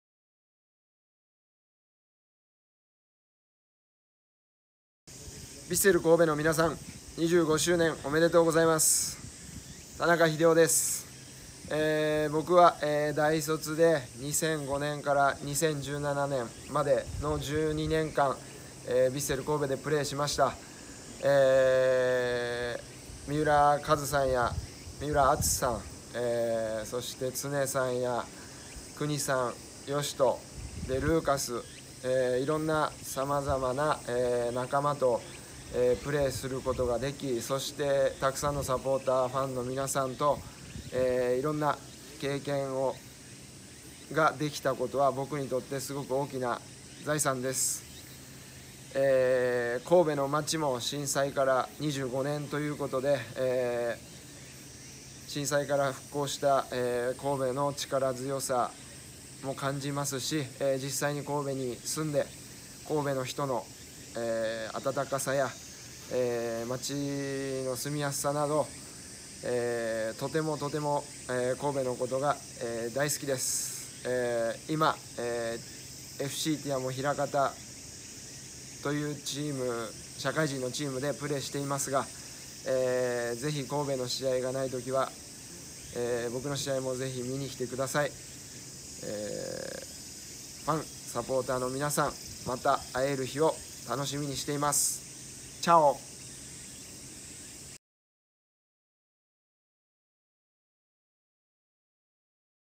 【25周年記念メッセージ】田中 英雄 氏｜HIDEO TANAKA
2005〜2014年、2015〜2017年に在籍していた田中英雄氏から【25周年記念メッセージ】をいただきました！

▼ヴィッセル神戸25周年記念特設サイト